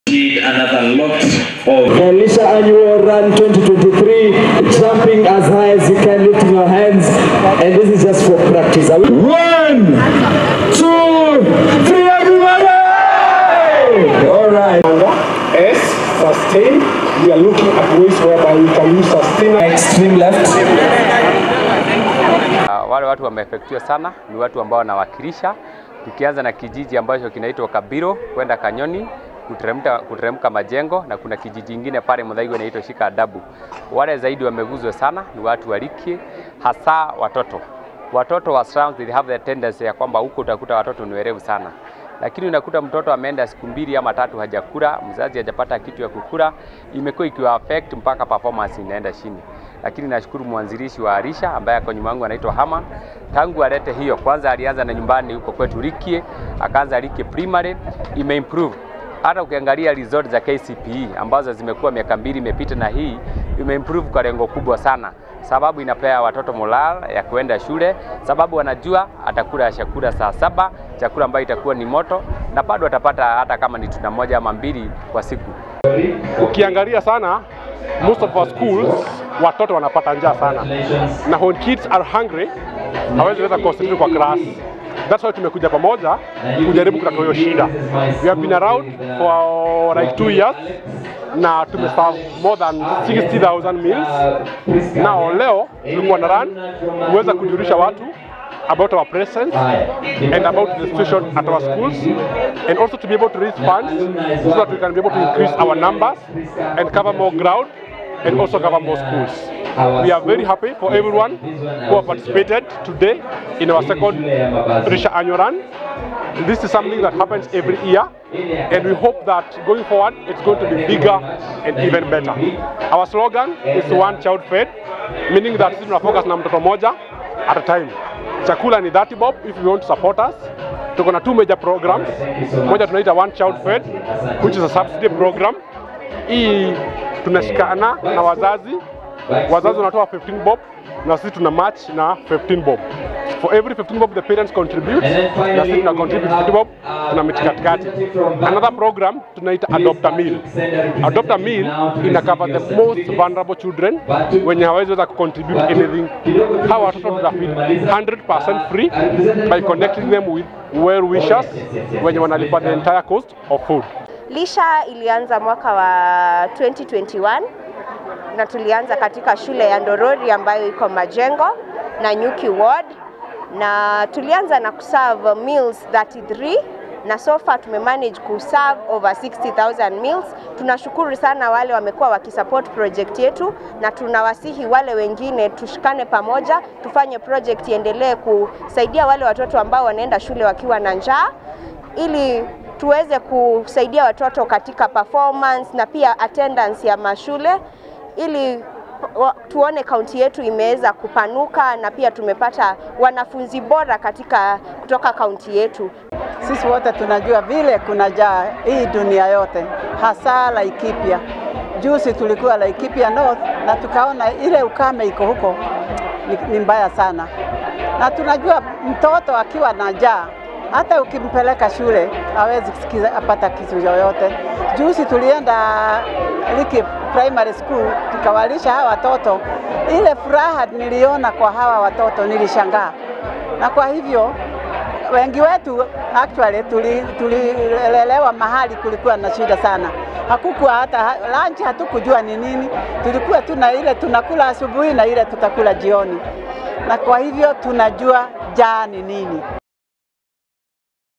Feeding slum children
A group of well-wishers from Nanyuki town has conducted a walk aimed at raising funds to feed school-going children from slum areas of Nanyuki town.

The group named Lisha project has been feeding learners from Majengo, Likii, Shika Adabu and other slum areas for two years now. Still, with the serious food shortage that has hit the country over the few months, they have received an increasing demand for food aid thereby resulting them to conduct this walk.